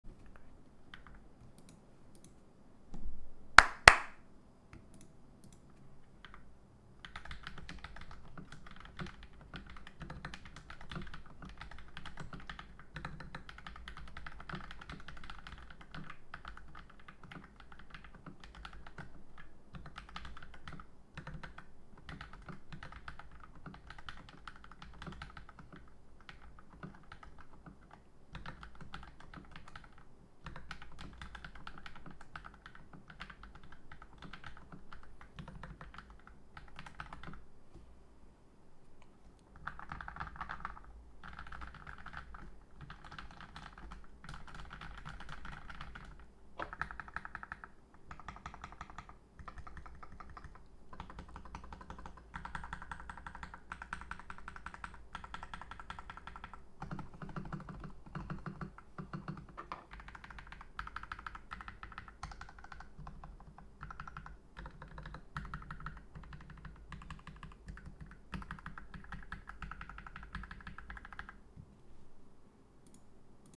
POPPY B68
Mods:
Lubed akko matcha
lubed and tape stabilizer